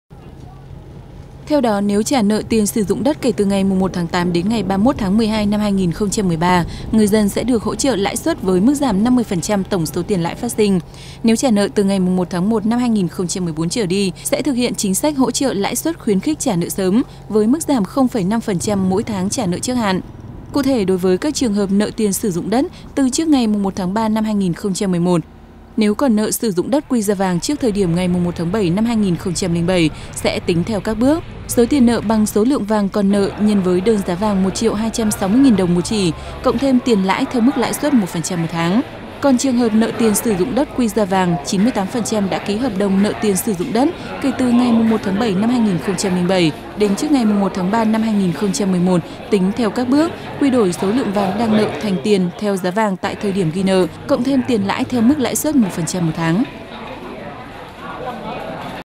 VTC14_Đà Nẵng áp dụng chính sách thu nợ tiền sử dụng đất
Theo thông tin của UBND TP Đà Nẵng, bắt đầu từ tháng 8/2013, địa phương này chính thức áp dụng chính sách thu nợ tiền sử dụng đất đối với các hộ gia đình, cá nhân được giao quyền sử dụng đất để làm nhà ở và cho nợ tiền sử dụng đất trên địa bàn.